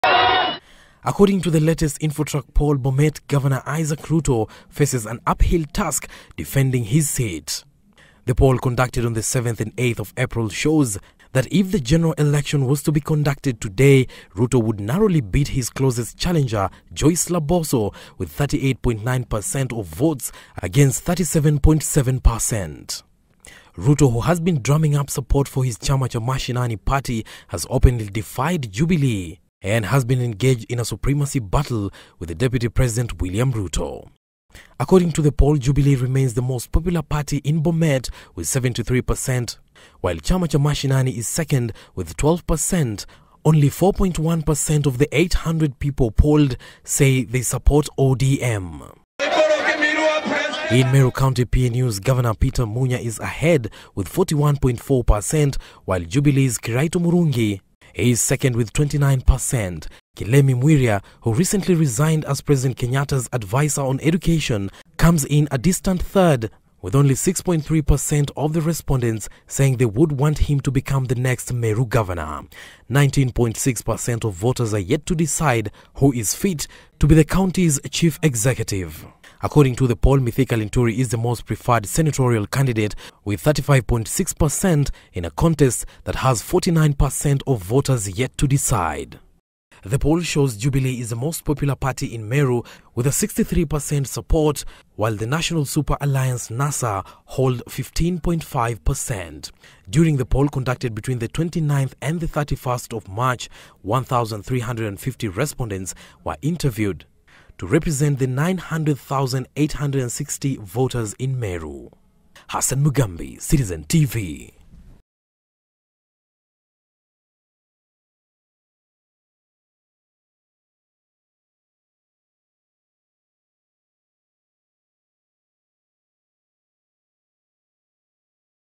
Infotrak poll puts Governor Munya ahead
A new infotrak poll indicates that Bomet Governor Isaac Ruto will have a hard time defending his seat from his closest challenger Joyce Laboso. Meru Governor Peter Munya however leads his political nemesis Kiraitu Murungi by 12 percentage points according to the poll released.